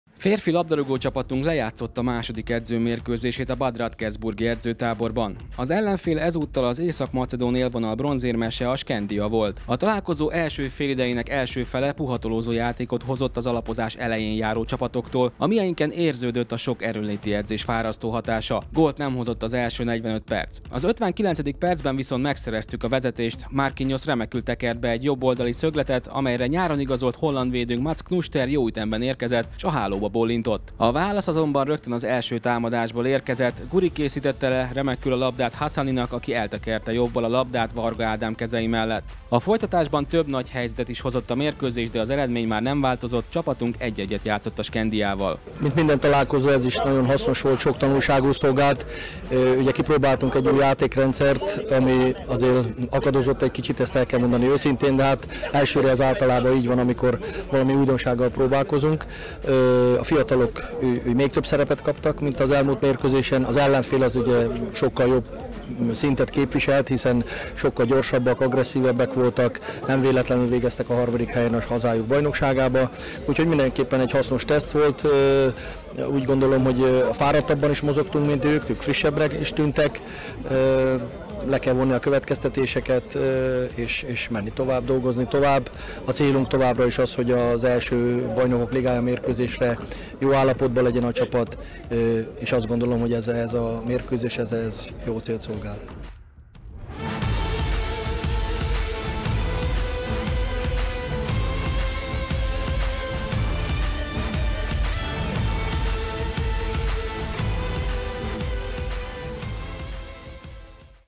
FM | „Célunk az első BL-selejtezőre készen állni” | 2022.06.12.
Máté Csaba szerint a Shkendija elleni 1-1-re végződött mérkőzés jól szolgálta felkészülésünket – VIDEÓ

Férfi labdarúgócsapatunk lejátszotta második edzőmérkőzését a Bad Radkersburg-i edzőtáborban. Az ellenfél ezúttal az északmacedón élvonal bronzérmese, a Shkendija volt. Az 59. percben szereztük meg a vezetést Mats Knoester fejesével, azonban rögtön az első támadásból érkezett a válasz Hasani révén. A folytatásban több nagy helyzet is volt a mérkőzésen, de az eredmény már nem változott, csapatunk 1-1-et játszott a Shkendijával. A találkozó után pályaedzőnk, Máté Csaba értékelte a látottakat.